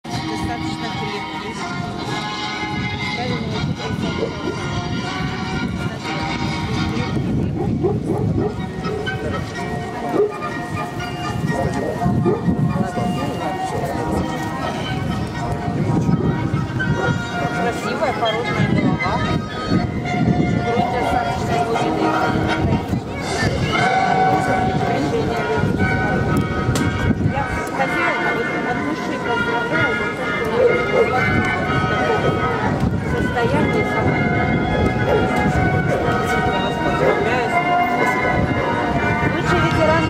KAZIMIR
Чемпионат по породе Среднеазиатская овчарка. Кировоград, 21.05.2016